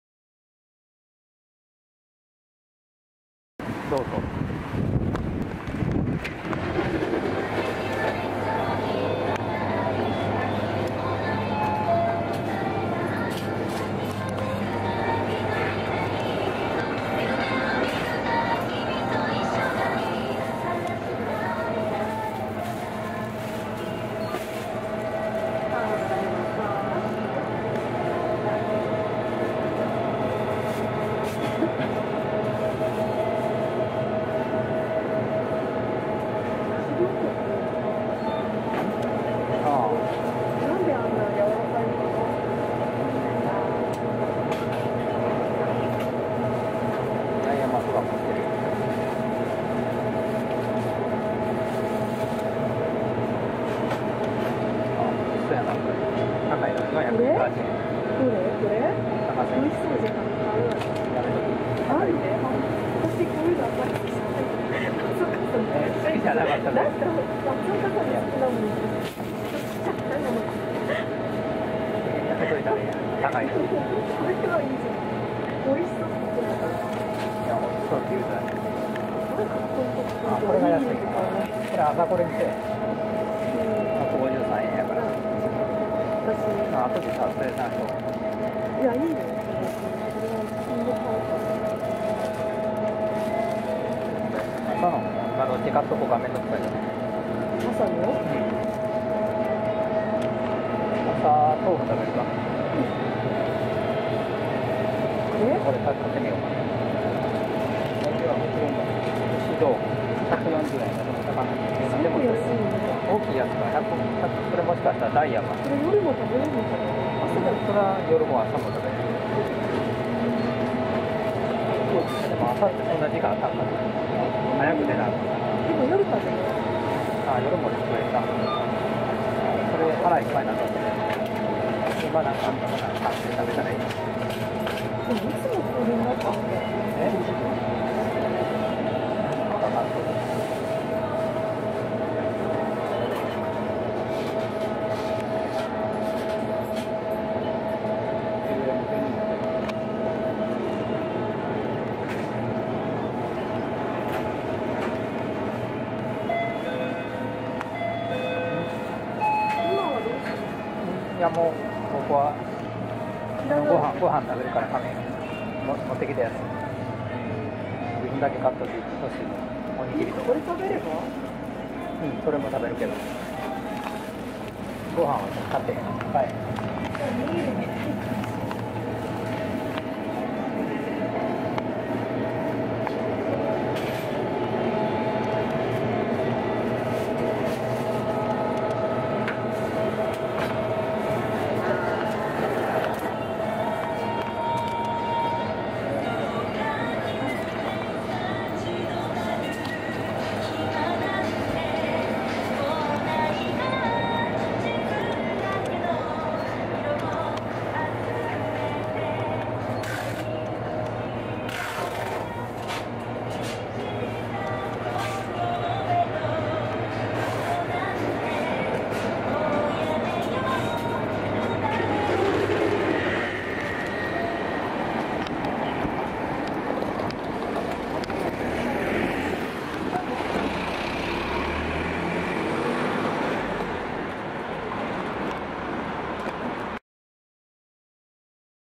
ファミマに買収された日本最古のコンビニCoco！に何があったのか？残存店舗石垣島店徹底取材の裏側！
20171014製作　秋蛇星短編映画製作所
 沖縄・八重山探検隊 2016 ～黒島のすべて（自転車島内一周散歩）と石垣島中心部 安宿エミックスと石垣市公設市場周辺、コンビニ探検隊（Coco!とファミマ）